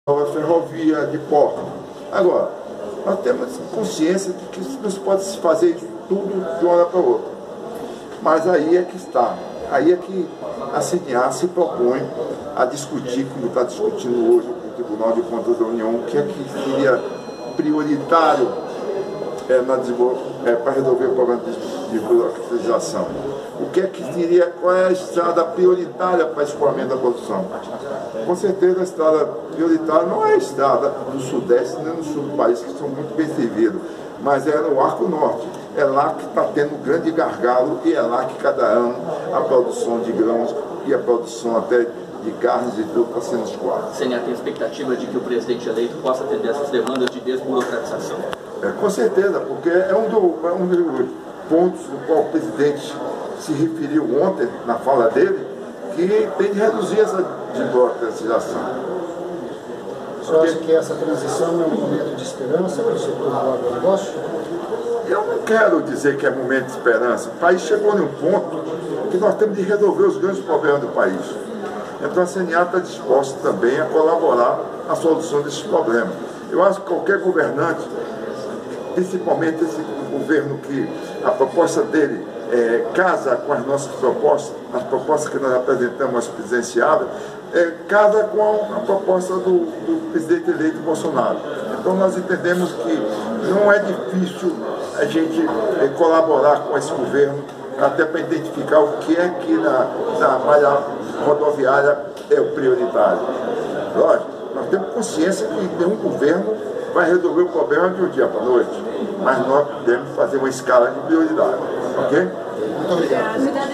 CNA: Propostas de Bolsonaro "casam" com as do setor agropecuário
Ao defender a desburocratização da máquina pública e investimentos em infraestrutura em benefício do produtor brasileiro, o presidente da Confederação da Agricultura e Pecuária do Brasil (CNA), João Martins, afirmou nesta terça-feira, dia 30, que o “Brasil chegou ao ponto da necessidade de resolver os problemas mais urgentes do país”. Ele cobrou ações e iniciativas governamentais para resolver os gargalos da produção no Arco Norte, disse esperar que o presidente eleito Jair Bolsonaro (PSL) possa começar a resolver os problemas. “Sabemos que não vai se resolver o problema da noite para o dia, mas as propostas [do presidente eleito Jair Bolsonaro] casam com as nossas. Ele disse, por exemplo, que é preciso reduzir a burocracia, é o que nós defendemos”, declarou, ao lembrar o Encontro com Presidenciáveis feito pela entidade durante a campanha eleitoral deste ano, com a entrega de propostas de compromisso com os então candidatos para fortalecimento do setor do campo brasileiro.